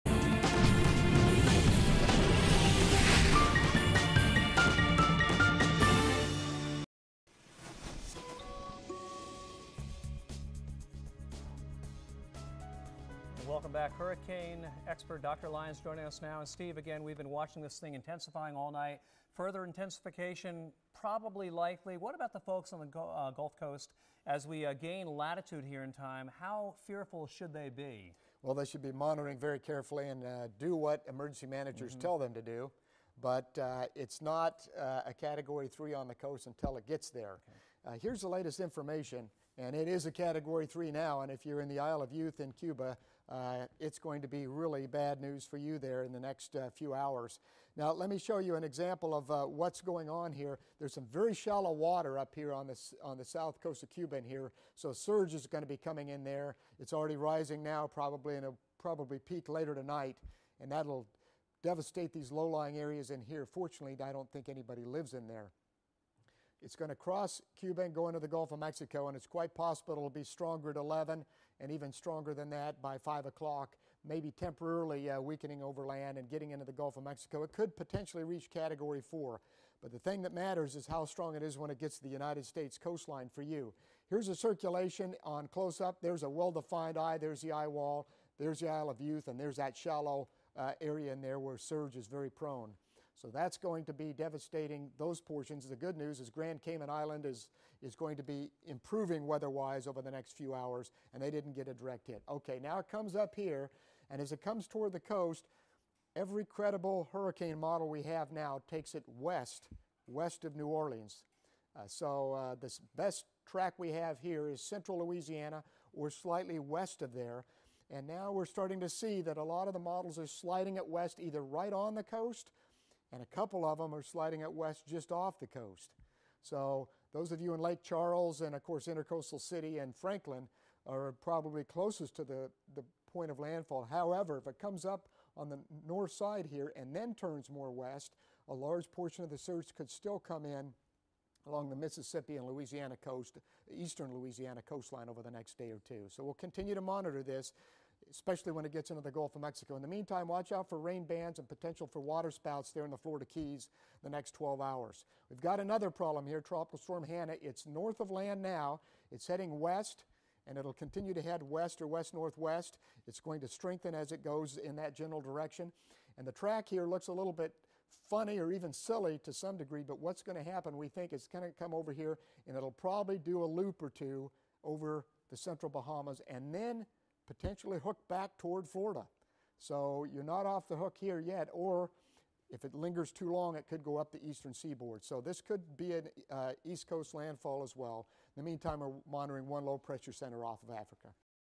Gustav Becomes a Major Hurricane
That's right, we have a major hurricane churning out there in the Caribbean, and it's only going to get stronger before it's all said and done. It's path takes it to the north Gulf of Mexico. All interests in that area need to keep tabs on this thing, that's for sure.